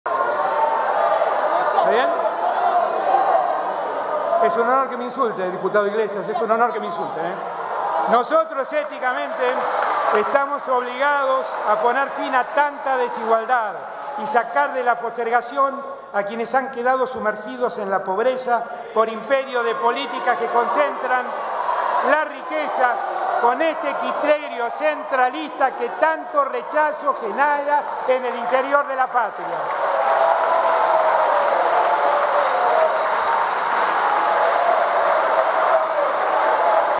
"Es un honor que me insulte, diputado", el duro cruce de Alberto Fernández con Fernando Iglesias
El discurso de Alberto Fernández en la apertura de las sesiones legislativas 2023 transcurría en un clima de tranquilidad, hasta que se refirió al conflicto con la Ciudad por la coparticipación. Entonces se desató una enardecida discusión con la oposición, mientras de fondo sonaban los aplausos de legisladores oficialistas.

"Es un honor que me insulte, diputado Iglesias", le dijo el mandatario a Fernando Iglesias, de Juntos por el Cambio, interrumpiendo por primera vez su discurso, que hasta ese momento había leído casi de corrido.